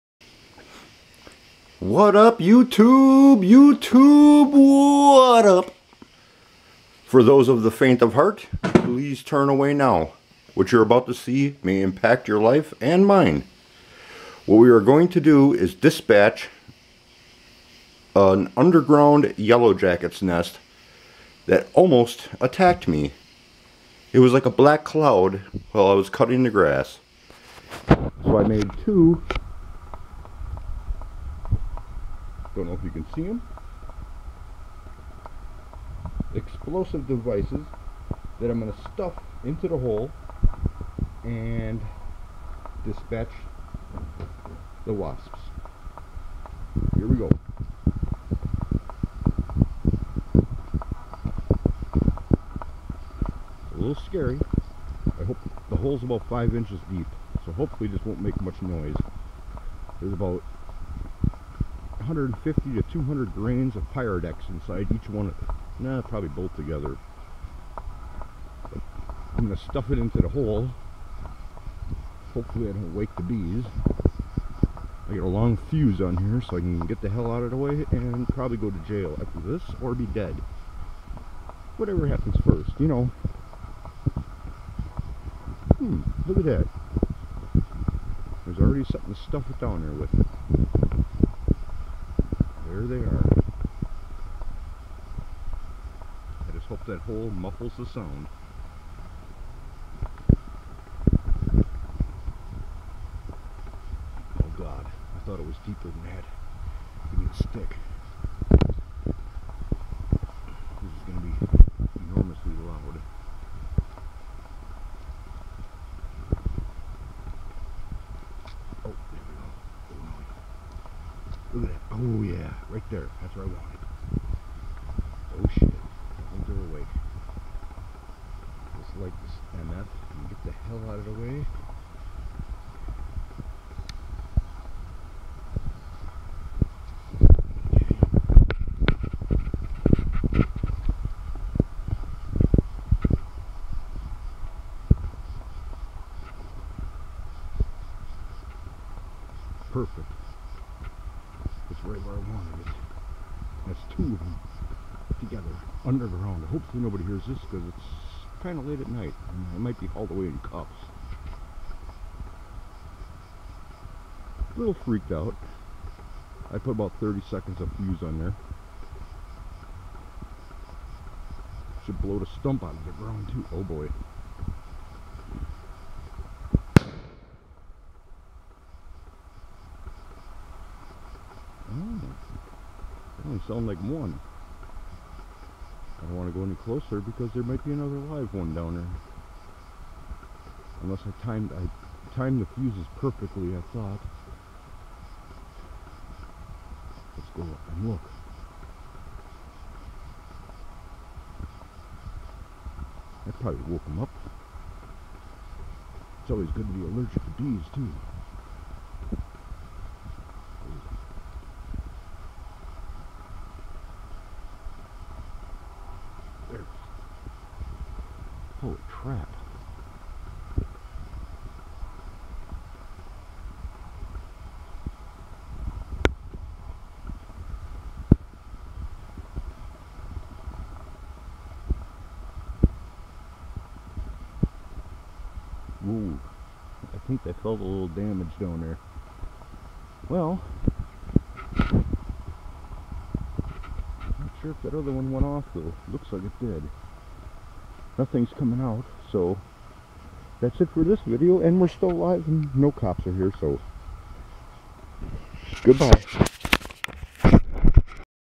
How to remove a yellowjackets nest the fun way!
I was mowing the lawn and after a few passes over the same area, I noticed a black cloud of yellow jackets! I didn't get stung but I am an amature pyrotechnic so I made some explosive devices to put down the hole at night and had 2 tied to go off at exactly the same time, The aftermath was no trace of any nest except a very charred honeycomb and not sure if both went off as planed as no trace was found after digging. Well, that did the trick!